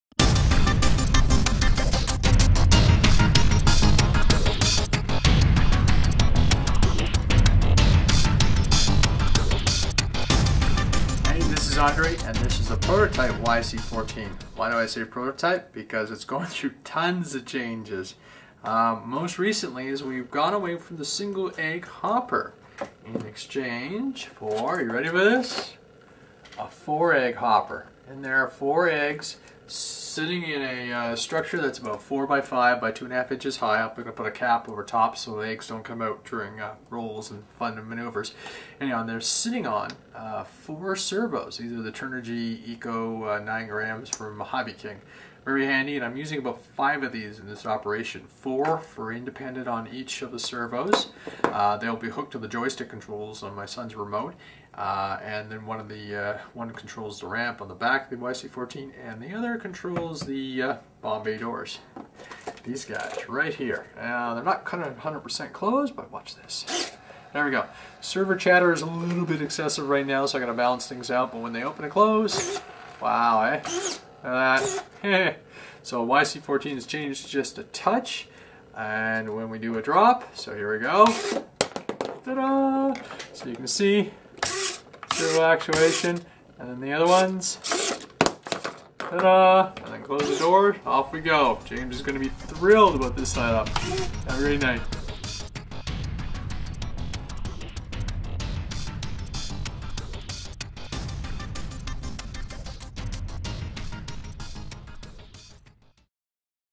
YC-14 Bomb Bay Doors
The YC-14 gets upgraded from a single egg hopper to a quad system with bomb bay doors